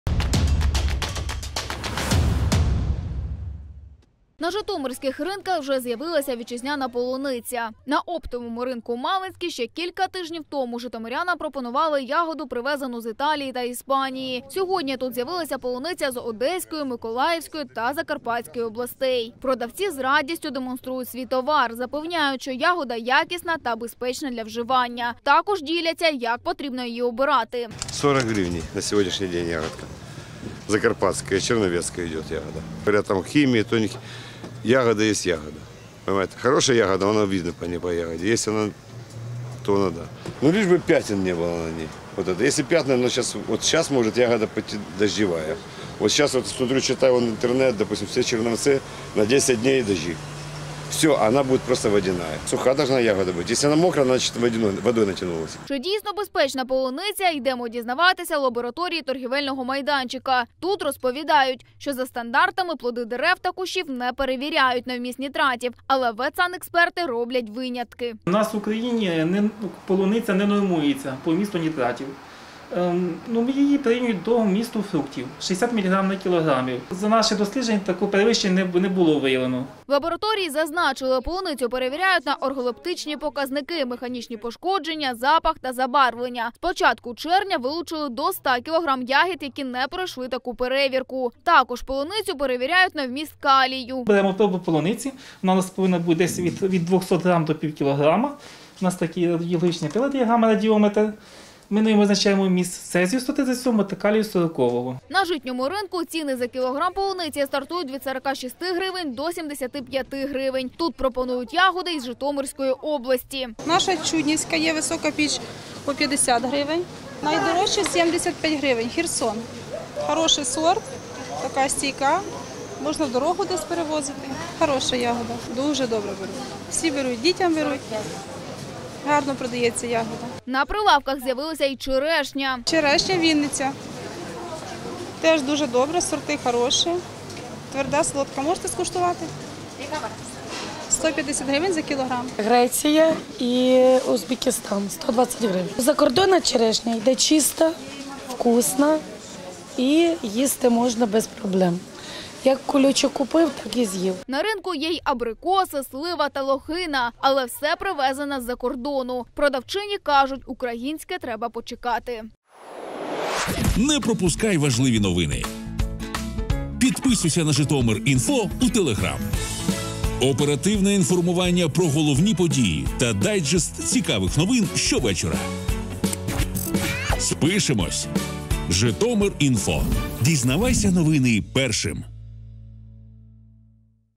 Яку полуницю пропонують на Житомирських ринках: звідки, скільки коштує та чи безпечна - Житомир.info
На житомирських ринках вже з’явилася вітчизняна полуниця.

На оптовому ринку «Малинський» ще кілька тижнів тому житомирянам пропонували ягоду, привезену з Італії та Іспанії, а сьогодні тут з’явилася полуниця з Одеської, Миколаївської та Закарпатської областей.

Продавці з радістю демонструють свій товар. Запевняють, що ягода якісна та безпечна для вживання. Також діляться, як потрібно її обирати.